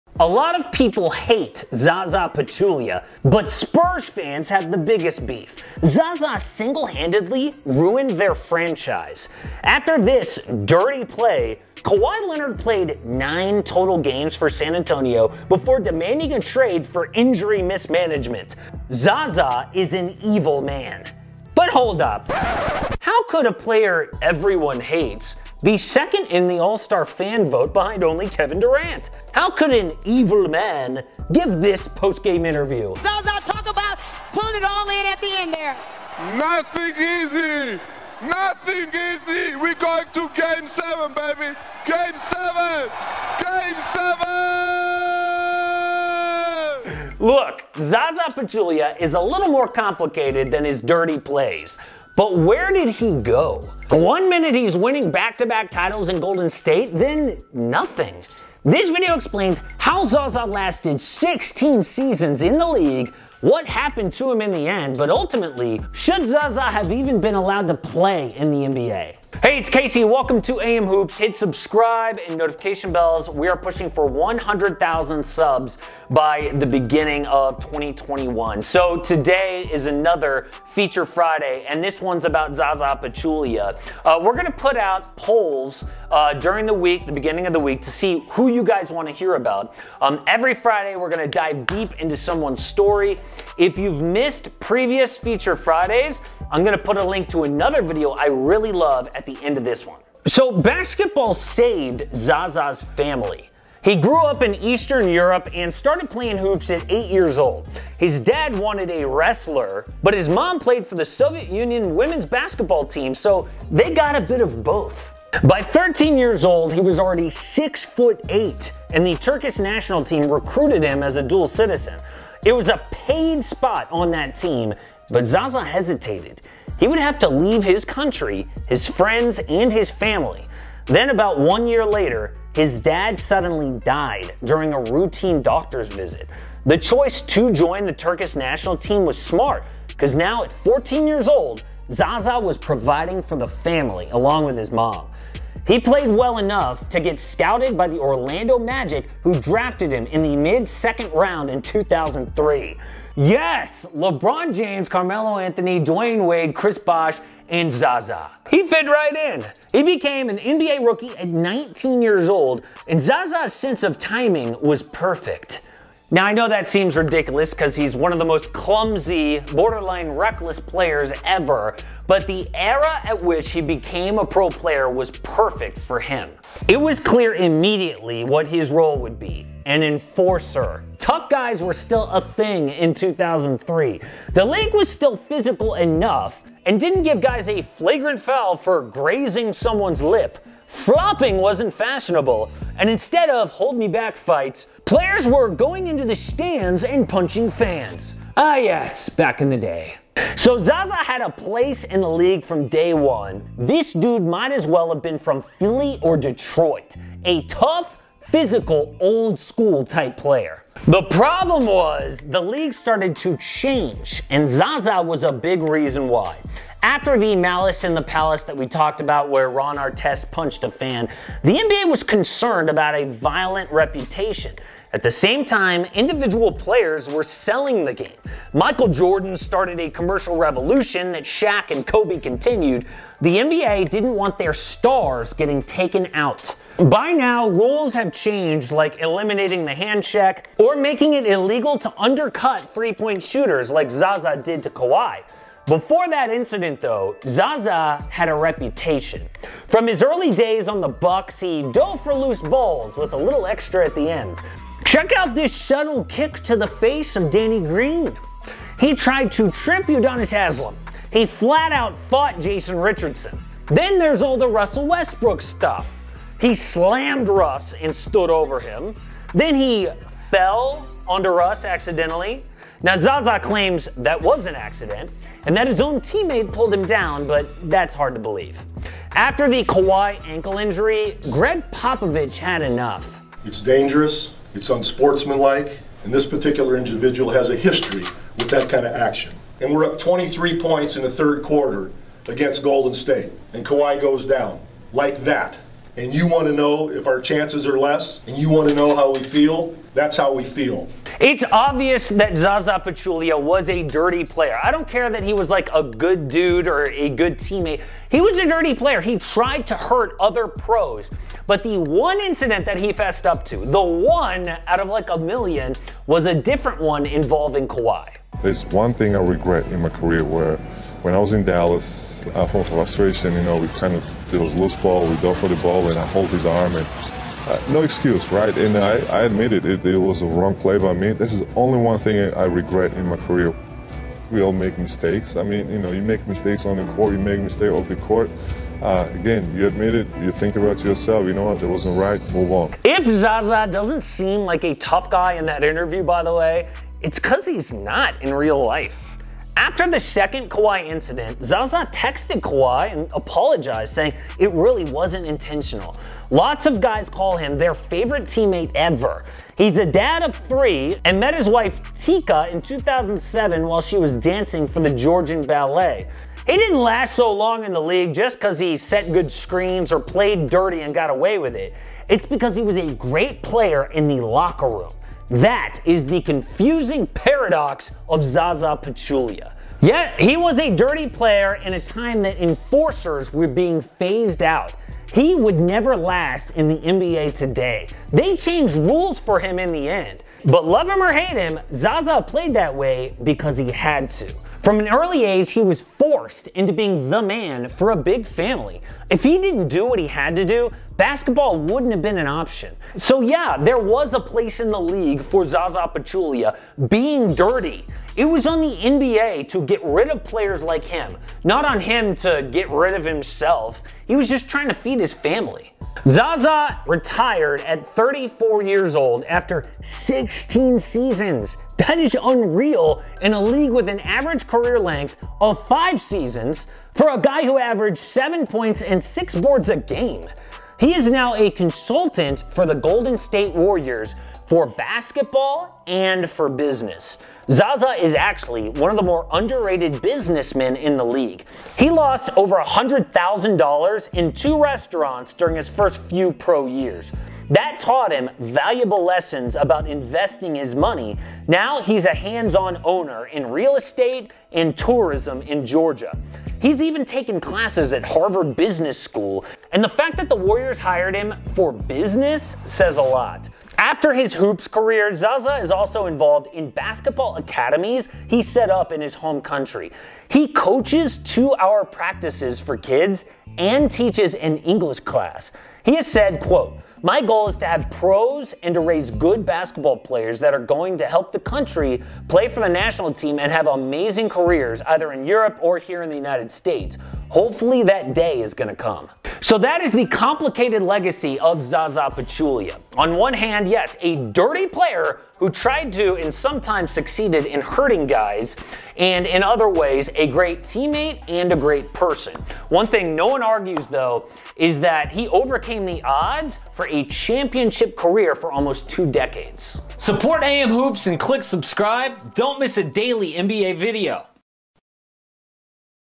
What happened to Zaza Pachulia? [DIRTIEST PLAYER IN DECADES]
Zaza Pachulia was known for his dirty plays during his 16 seasons in the NBA. He is controversial for good reason but Pachulia is more complicated than just his dangerous moments on the court. This video explains why Zaza last as long as he did despite being an average talent and what eventually happened to Zaza Pachulia.